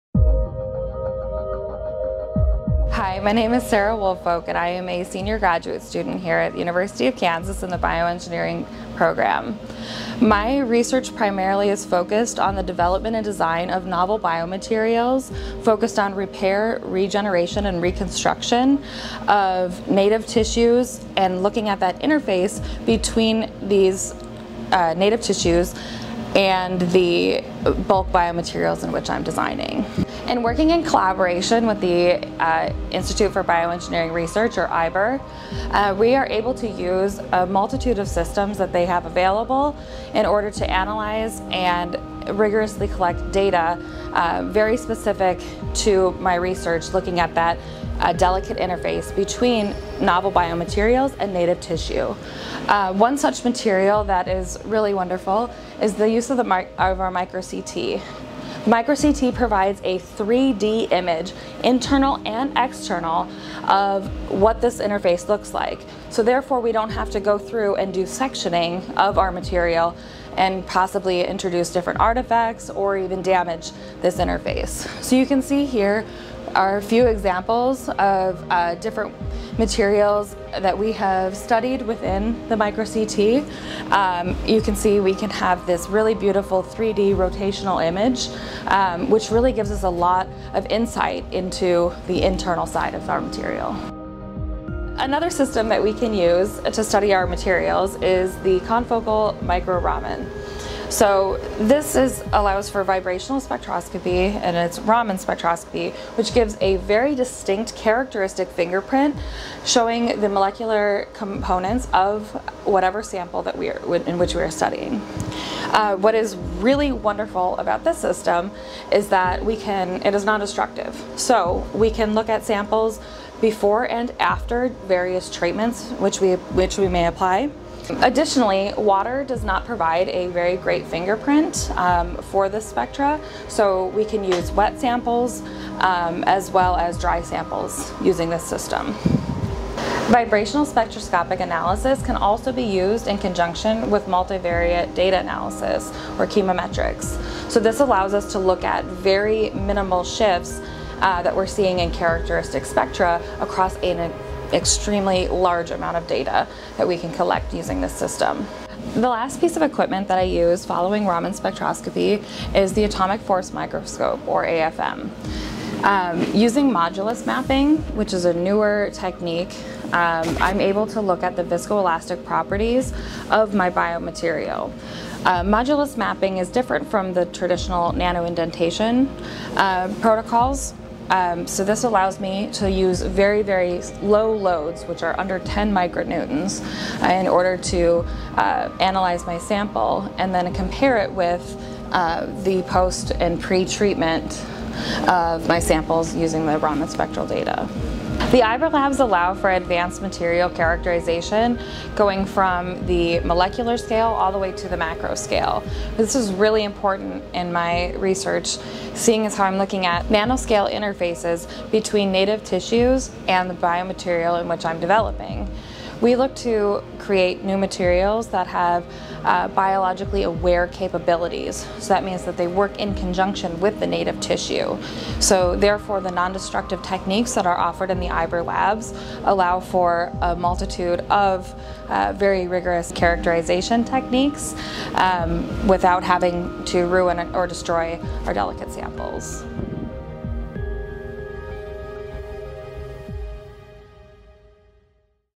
Advanced Biomaterials and Biointerfaces Lab
Analytical capabilities in the Advanced Biomaterials and Biointerfaces lab are used to correlate structural organization, i.e., chemical, physical and morphological features with macroscopic properties (elastic modulus, toughness, elongation and fracture). Protocols are optimized to provide nondestructive, in situ multi-scale (molecules to fractions of meters) structure/property characterization of soft and hard tissues, materials and material/tissue interfaces.